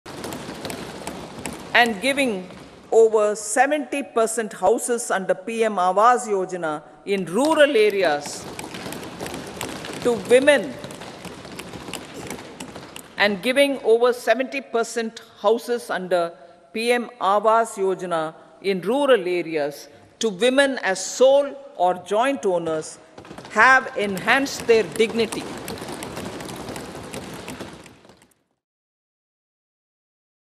We are about to achieve the target of 3 Cr houses
“Implementation of PMAY-G continued despite the obstacles of Covid19 and we are about to achieve the target of 3.0 Cr houses.” – Hon’ble Minister of Finance and Corporate Affairs Nirmala Sitharaman